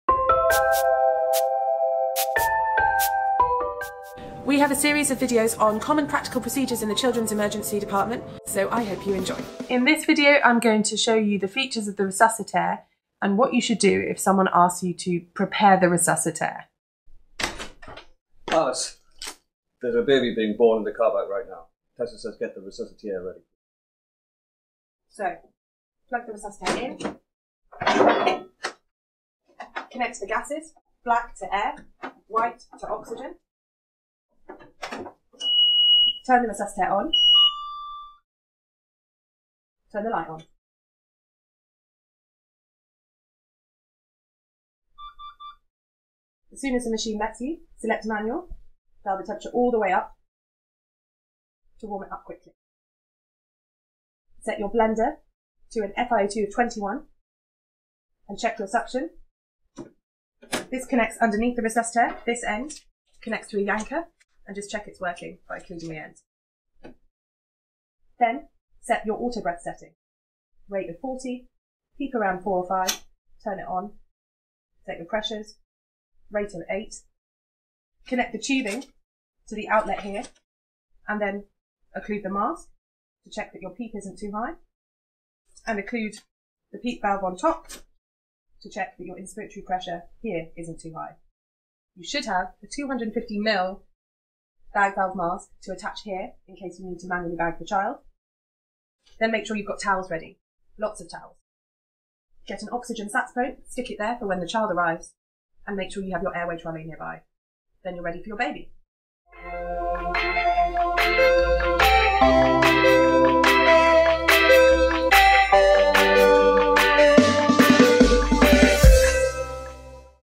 Resuscitaire run through
There's a baby being born - get your resuscitaire ready and primed.